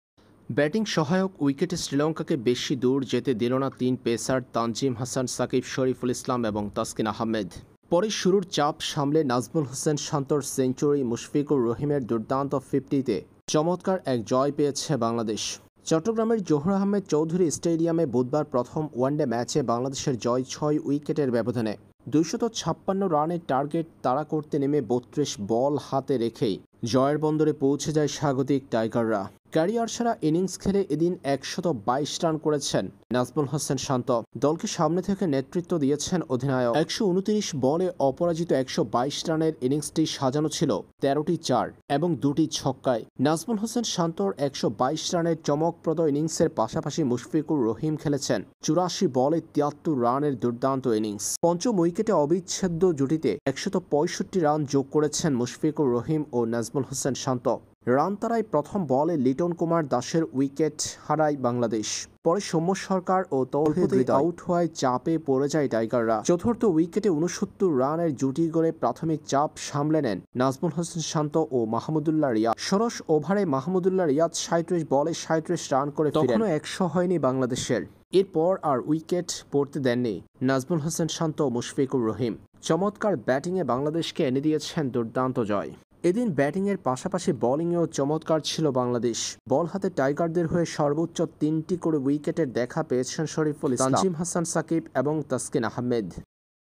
শান্তর সেঞ্চুরি, শ্রীলঙ্কাকে নাগিনের বিন বাজিয়ে নাচালো টাইগাররা | Bangladesh vs Sri Lanka | Shanto
Bangladesh vs Sri Lanka 1st ODI Match 2024 - Nazmul Hossain Shanto century today vs sri Lanka in 1st ODIs in chattagram. / Bangladesh National Cricket team wins by 6 wicket against Sri Lanka in first ODI match, 3 match ODI series 2024 | taskin Ahmed, shariful Islam Tanzim hasan shakib takes 3 wicket /

শান্তর সেঞ্চুরি, শ্রীলঙ্কাকে নাগিনের বিন বাজিয়ে নাচালো টাইগাররা | Bangladesh vs Sri Lanka | Shanto century